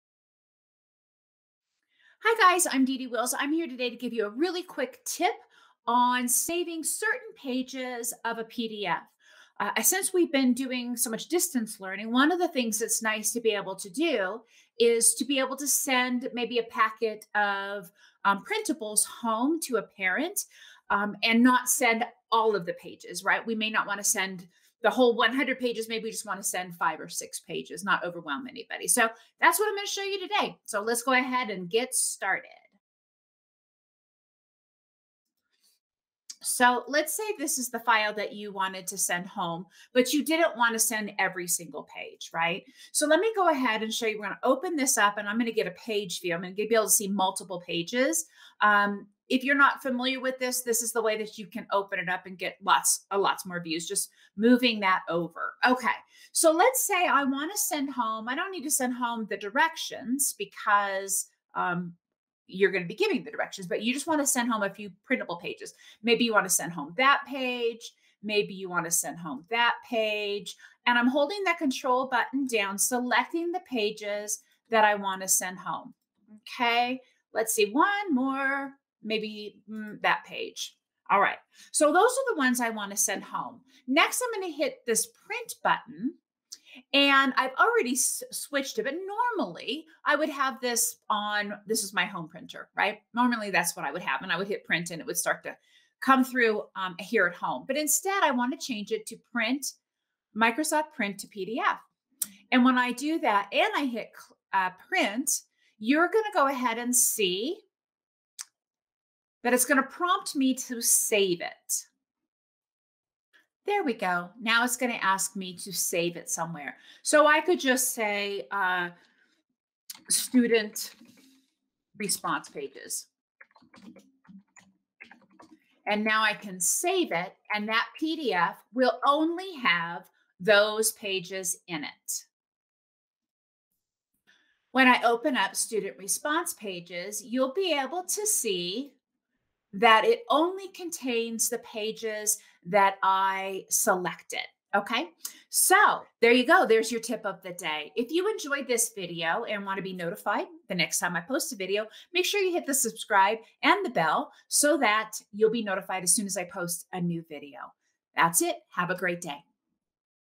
How to save just a few pages from a PDF file
How to save just a few pages from a PDF file.   Maybe you have a 200-page document, but you want to be able to save just a few pages.  Let me show you how.

Teachers may want to send just a few pages home to a student.  This video will show you how to extract the exact number of pages you want and save them as an individual  PDF... even if they are a secured PDF.

Here is a link to the file I was using to walk you through the steps.